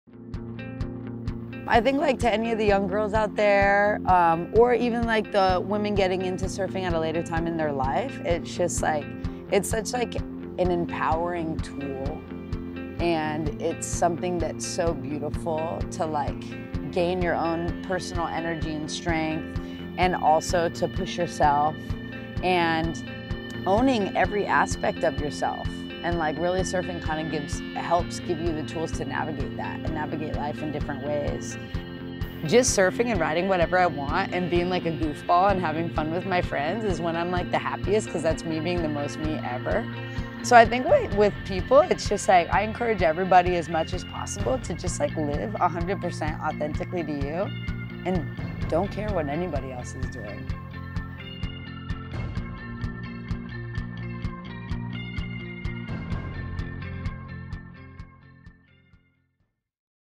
Kassia Meador Is Thrilled About the Future of Women's Surfing - The Inertia
Surfer Kassia Meador shares her perspectives as a leader in the surf community and the exciting opportunity facing women today.

“To any of the young girls out there or even to women getting into surfing at a later time in their life, it’s such an empowering tool,” said Kassia Meador. “It’s something that’s so beautiful to gain your own personal energy and strength and push yourself and own every aspect of yourself, and surfing helps give you the tools to navigate that and navigate life in different ways.”